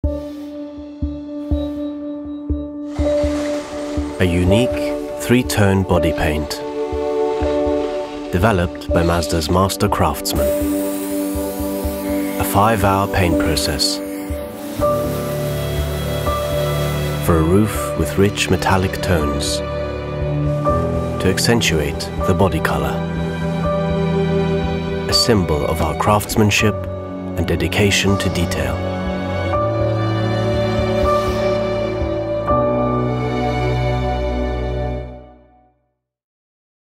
Úplne nová Mazda MX-30 - farba, ktorá inšpiruje
Naša nova, plne elektrická Mazda MX-30 má jedinočnú trojtónovú farbu karosérie. Pozrite sa na našich majstrov a ich umeleckú prácu pri lakovaní novej Mazdy MX-30. Tento proces trvá až 5 hodín.